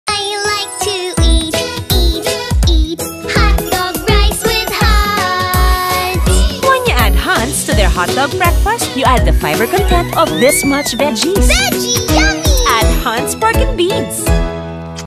Add Hunt’s Pork & Beans to your kid’s hotdog breakfast!
What’s yummy and healthy for breakfast? 😋
Veggies! Just like Hunt’s Pork & Beans! ❤️ Don’t forget to add it to your kid’s favorite ulam for breakfast. Coz it’s the yummy veggie na magugustuhan nila! ☀️